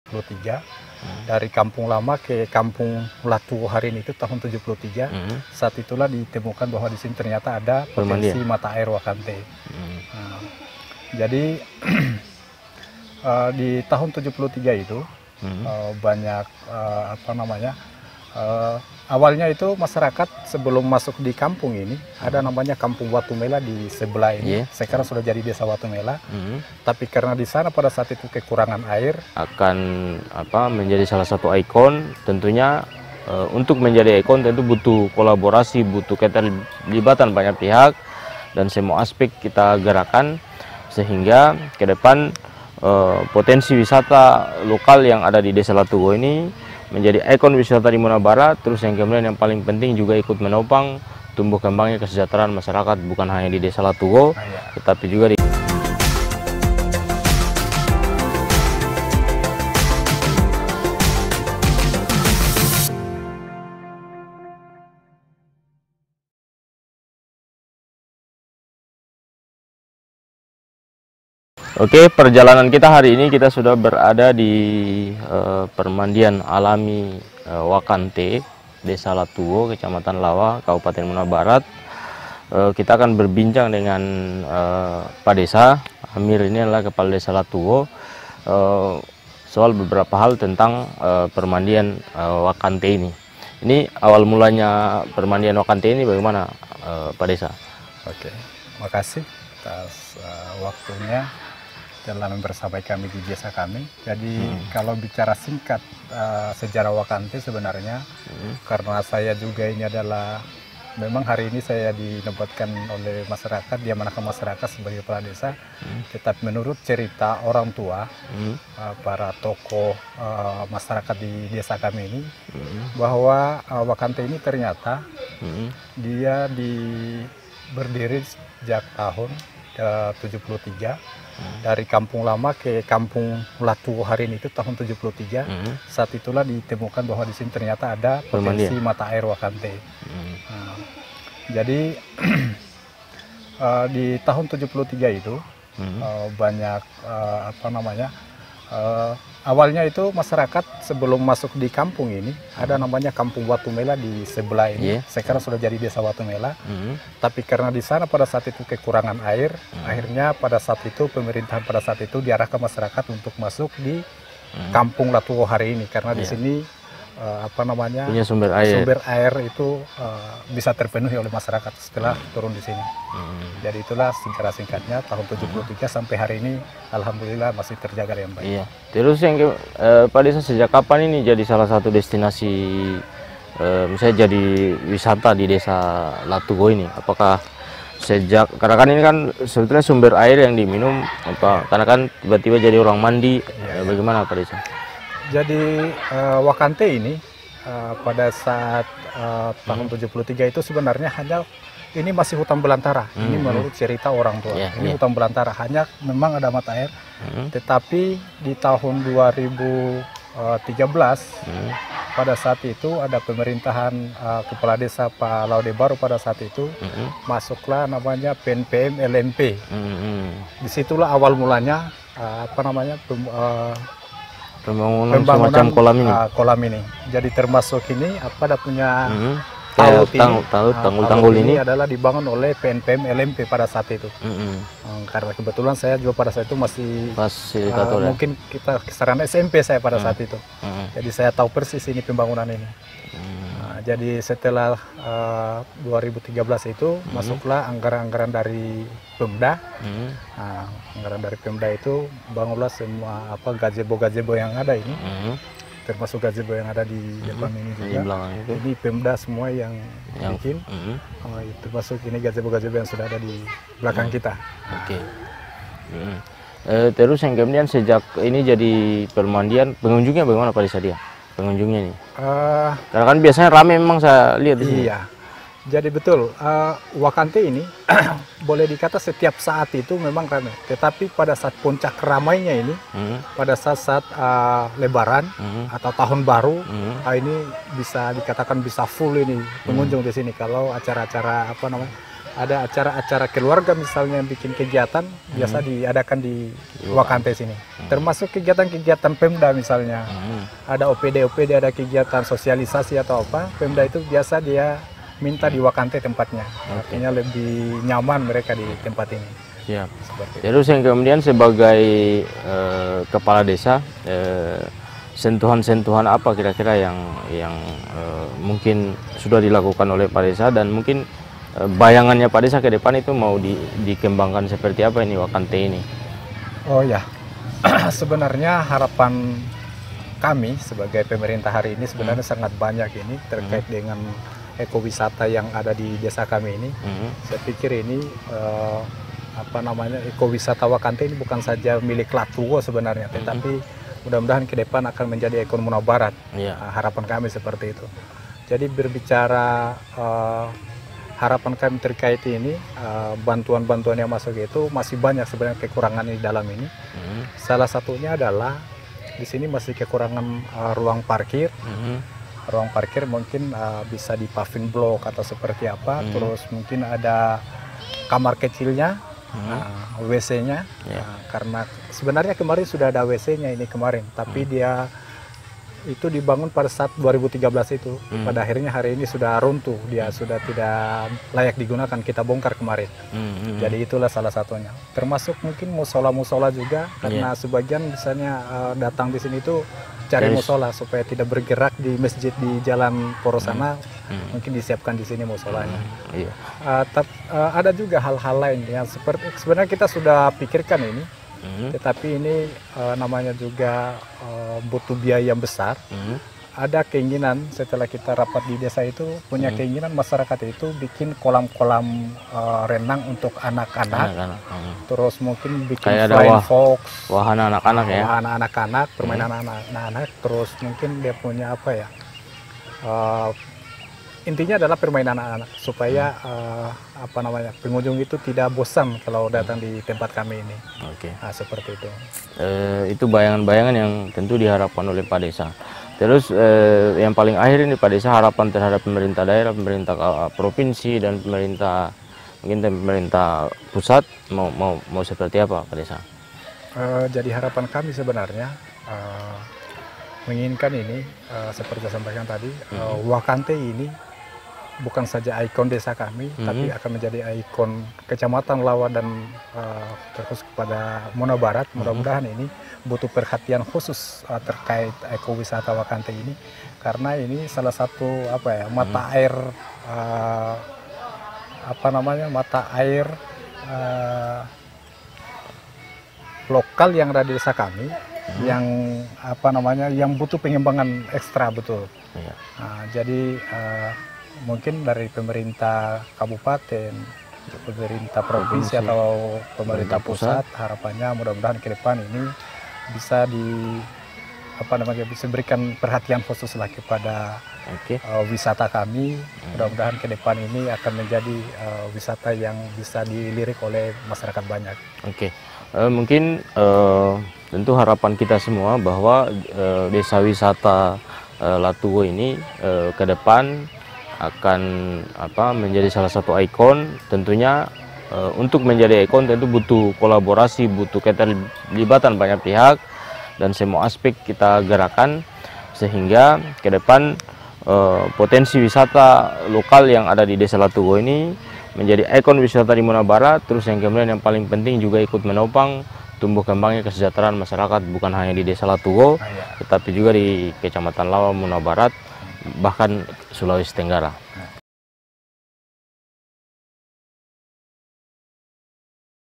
Keren‼️Permandian Wakantee, Wisata Permandian yang Masih Alami
Perjalanan Bang Jay kali ini sudah berada di permandian alami Wakante Desa Latugho Kecamatan Lawa Kabupaten Muna Barat. Bang Jay akan berbincang dengan Kepala Desa mengenai awal mula permandian Wakante dan pengembangannya kedepan. "Kali ini ini kita akan berbincang dengan kepala desa soal beberapa hal tentang permandian Wakantee ini. Bagaimana awal mulanya pembangunan permandian Wakate ini? “ tanya Bang Jay

Kepala Desa Latugho menjelaskan, tahun 1973 masyarakat dari kampung lama diarahkan untuk pindah di tempat yang dekat dengan mata air. “Kalau bercerita sejarah singkat permandian Wakante ini bahwa ditemukan pada tahun 1973. Jadi di tahun itu banyak masyarakat kampung lama bermigrasi atas arahan pemerintah desa untuk masuk di Desa Latugho. Sejak tahun itu hingga sekarang masih terjaga dengan baik." ujar Pak Desa

Wakante menjadi sumber air minum dan sekarang sudah dijadikan permandian. Kepala desa mengatakan Permandian Wakantee ini murni sebagai tempat permandian. “Jadi Wakantee ini waktu masih hutan belantara masih diminum. Namun pada tahun 2013 pada masa pemerintaha La Ode Baru sebagai Kepala Desa, disitulah awal pembangunan kolam dan talud Permandian Wakante. “ujarnya

Pada tahun 2013 anggaran sudah mulai menyentuh permandian ini termaksud pembanguna gazebo. Pak Desa menjelaskan pengunjung yang berwisata di Permandian Wakante ini sangat banyak mulai dari masyarakat biasa hingga pemerintah Kabupaten Muna Barat berwisata di permandian ini. “Jadi betul Permandian Wakante ini boleh dikata setiap saat ini ada pengunjung namun puncak ramainya ini pada saat tahun baru atau lebaran sangat banyak yang berwisata disini, apalagi Pemerintah Daerah Muna Barat selalunya berkegiatan di permandian ini." ujar Kepala Desa

Lebih lanjut Kepala Desa Latugho mengatakan pengembangan kedepan objek wisata Permandian Alam Wakantee ini diharapkan menjadi icon Muna Barat. “Sebenarnya kami berharap sangat banyak apalagi soal ekowisata, karena kami harapankan dapat menjadi icon Muna Barat. Dan kami juga berharap uluran tangan pemerintah untuk membangun lokasi parkir, toilet, dan tempat ibadah, karena banyak pengunjung yang ingin sholat, dan masih banyak juga hal-hal yang lain.“ ujarnya

Pembangunan wahana untuk anak anak juga menjadi prioritas pemerintah desa. Kepala Desa Latugho mengatakan bahwa potensi mata air Wakantee ini sangat baik untuk dikembangkan sehingga Permandian Wakante semakin banyak pengunjung. “Harapanya mudah-mudahan kedepan pemerintah kabupaten, pemerintah provinsi, dan pemerintah pusat dapat memberikan perhatian khusus kepada objek wisata permandian alami Wakantee ini." tututpnya

Tetap Ikuti video Bang Jay Lebih Dekat dengan Masyarakat Desa.